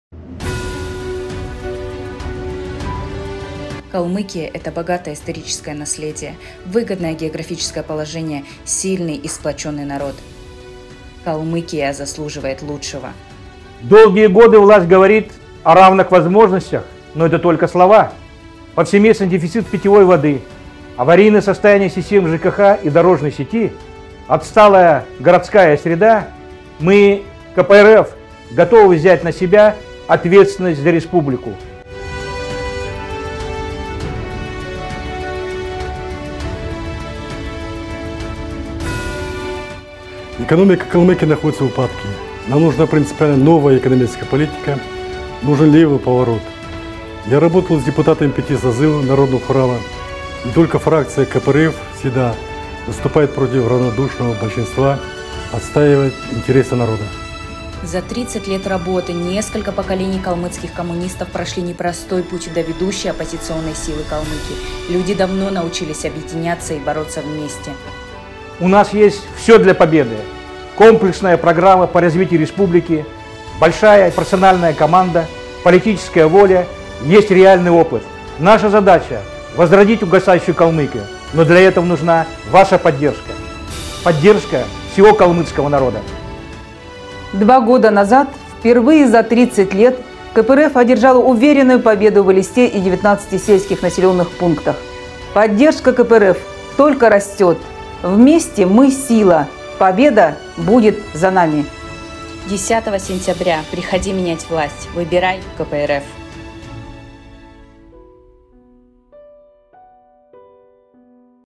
ВЫБОРЫ депутатов НАРОДНОГО ХУРАЛА - 2023. Ролик Калмыцкого республиканского отделения партии КПРФ
Меньше недели остаётся до начала народного голосования жителей Калмыкии за кандидатов в депутаты, которые займут кресла в новом 7-м созыве Народного Хурала.

На участие в предстоящих выборах - 8,9 и 10 сентября Калмыцкое республиканское отделение КПРФ выдвинуло кандидатов по всем 17-и одномандатным округам. В каждом округе, напомним, будет избран только один депутат. В парламенте Калмыкии он войдет в состав фракции той партии, которая его выдвинула.

Общереспубликанский список кандидатов КРО КПРФ возглавляют: под номером один - Нуров Николай Эрднеевич - член ЦК КПРФ, первый секретарь Комитета КРО КПРФ, заместитель Председателя Народного Хурала. Под вторым номером - Эрендженов Петр Валентинович - Герой Калмыкии, заслуженный экономист Российской Федерации, 20 лет возглавлявший Контрольно-счетную палату РК, признанный одним из лучших финансовых контролеров России. Тройку лидеров общереспубликанского списка КРО КПРФ замыкает Бембеева Мигмир Гаряевна – депутат Народного Хурала, заместитель главы фракции КРО КПРФ, кандидат в члены ЦК, секретарь Комитета КРО КПРФ, помощник депутата Госдумы.

Напомним, что согласно жеребьевке партия КПРФ будет идти под № 2 в избирательном бюллетене.